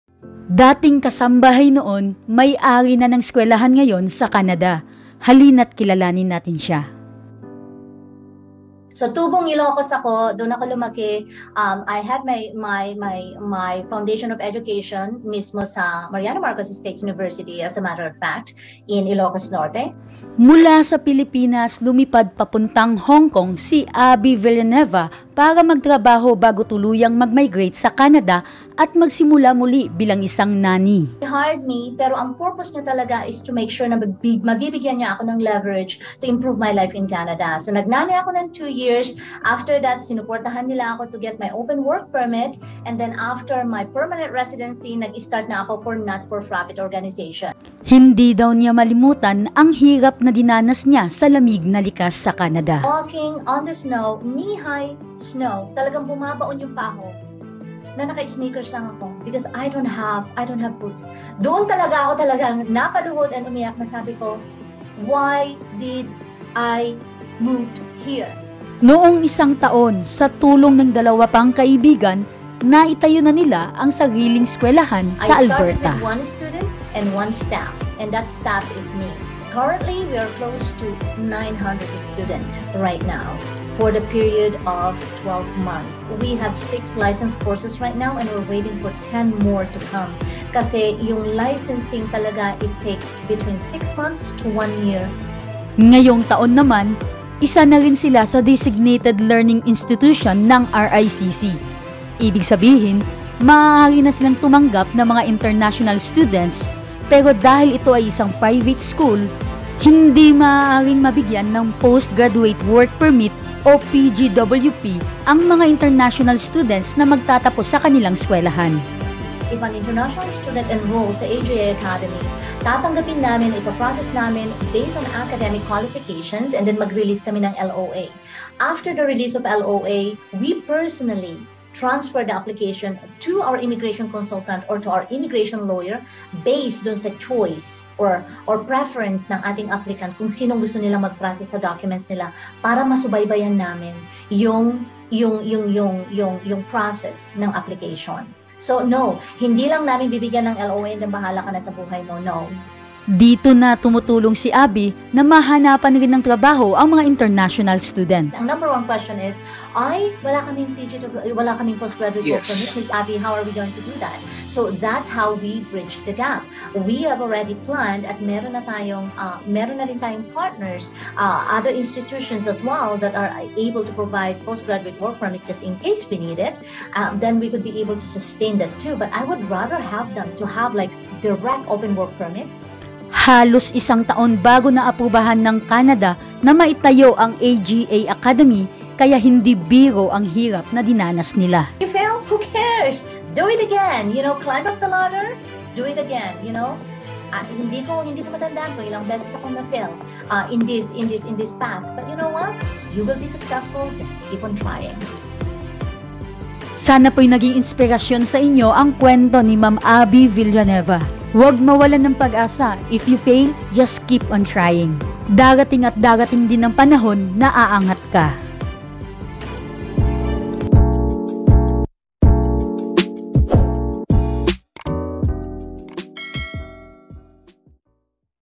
INSPIRATIONAL STORY: DATING KASAMBAHAY, MAY-ARI NA NG ESKWELAHAN NGAYON SA CANADA
This post is created for information, education, and dissemination purposes only.